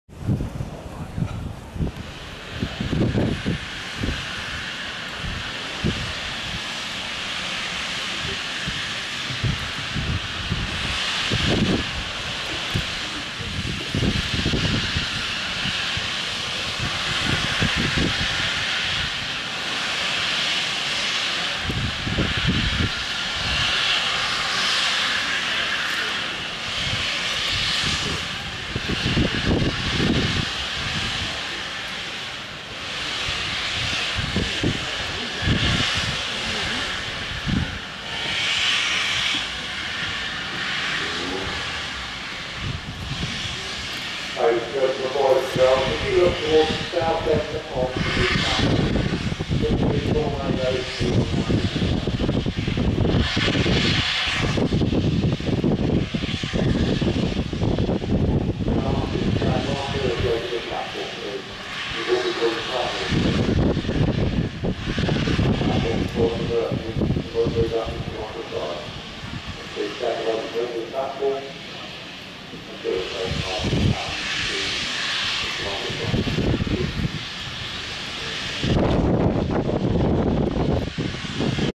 A1 Pacific 60163 Tornado moves out of Sheffield Park yard 10-09-13
60163 Tornado moves out of Sheffield Park yard on The Bluebell Railway in preparation for reattaching to the Cathedrals Express charter which had arrived behind Tornado earlier in the day. The return run would be diesel hauled as far as South Croydon where Tornado would take charge once again for a great run over the Surrey Hills. A very enjoyable day and a great performance from Tornado and her crews.